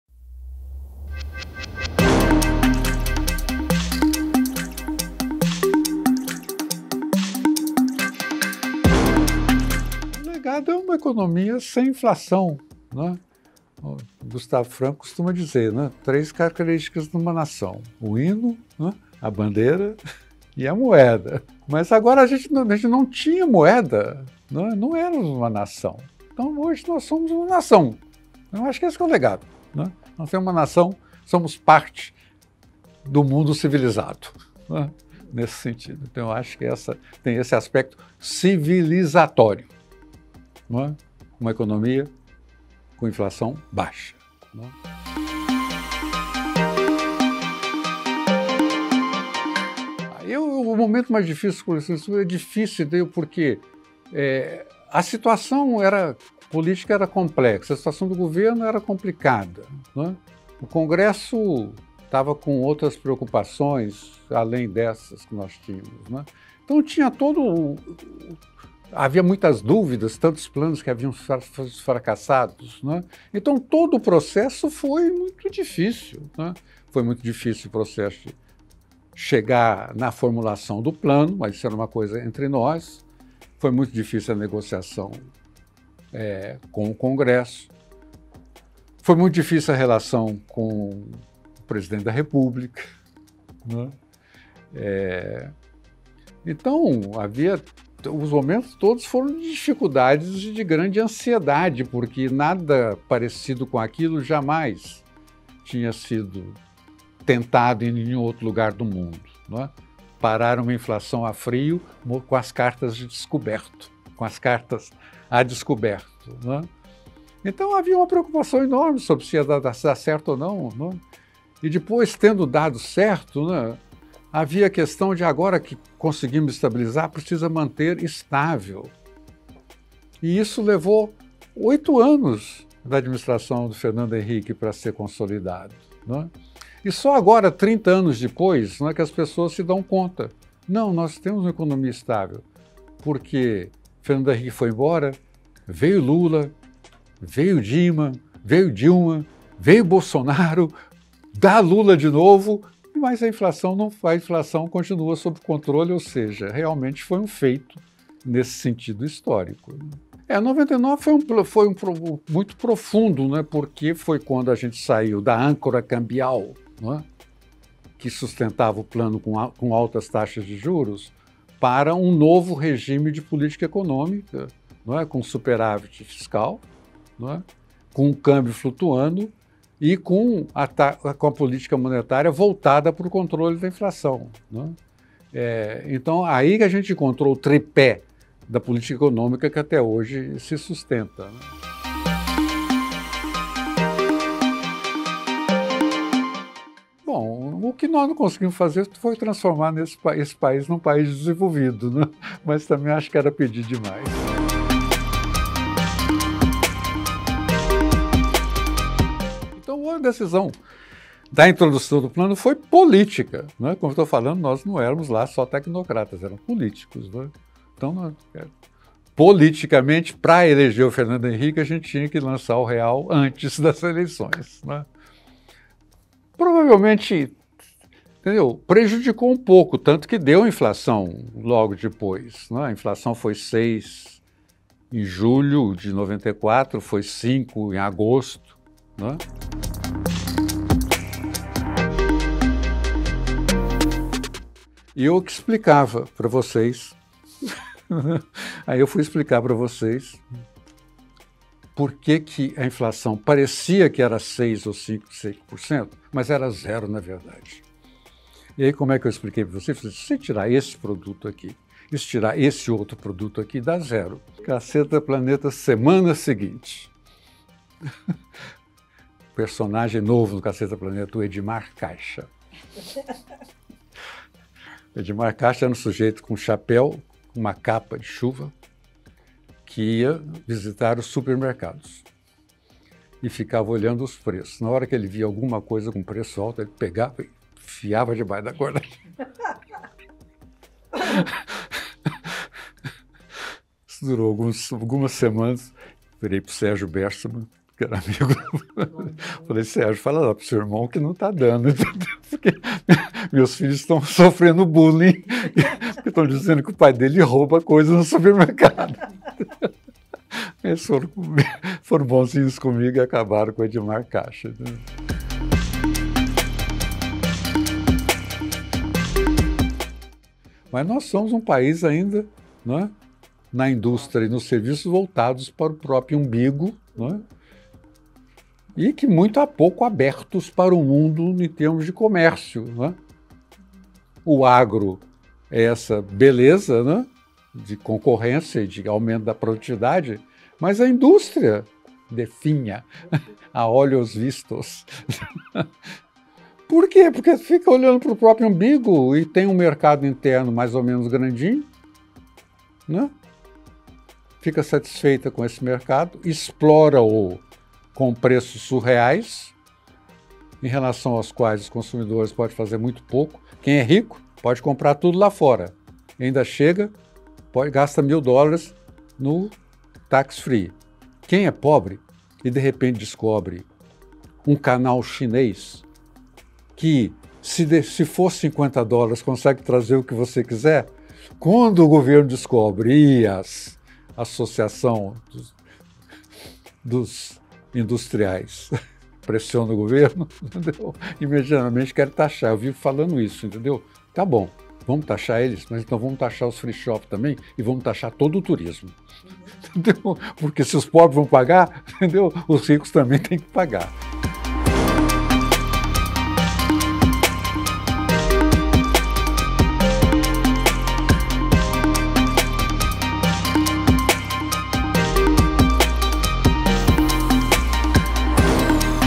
30 anos do Plano Real: Edmar Bacha
Economista da equipe que idealizou e implantou a nova moeda lembra momentos marcantes